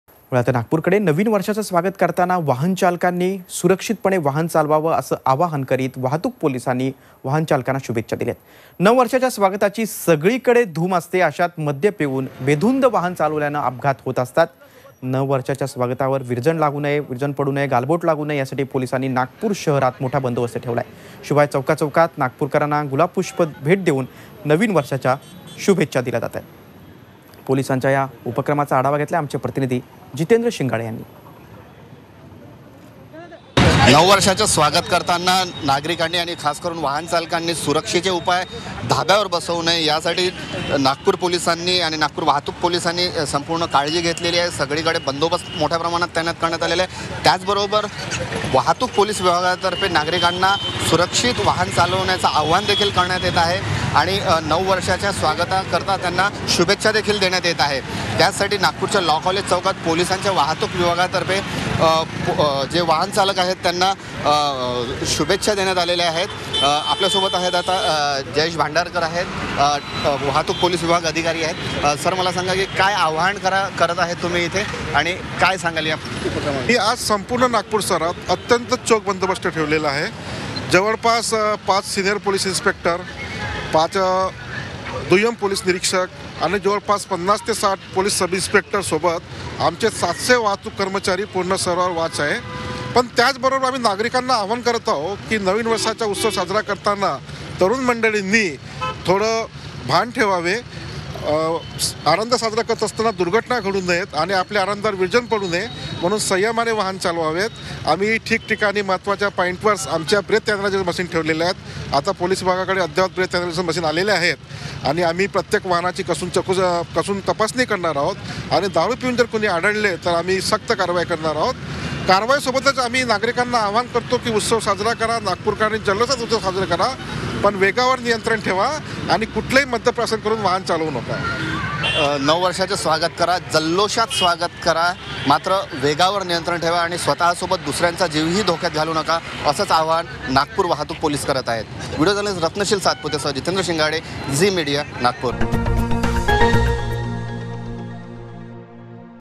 नागपूर | वाहनचालकांनो सुरक्षितपणे वाहन चालवा!
Nagpur Police gives massage of safe driving to all drivers by giving rose